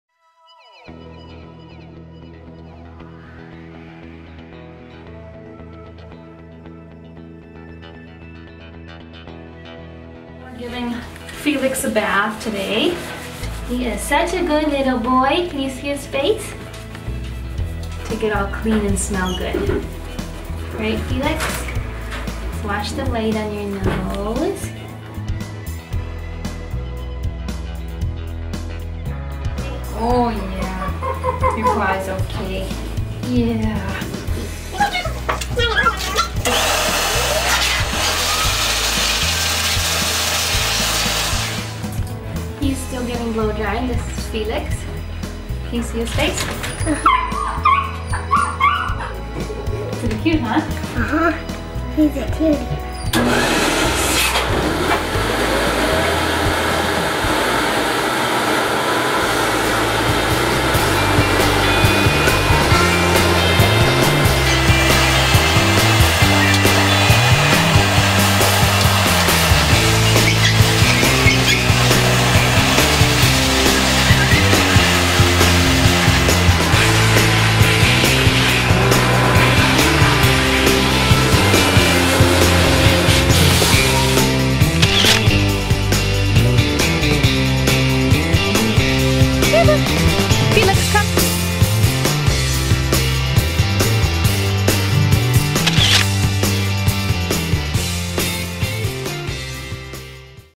F1 Standard Bernedoodles
"Felix" - Proud parents are Tobias x Paisley. We offer well socialized bernedoodles to forever families.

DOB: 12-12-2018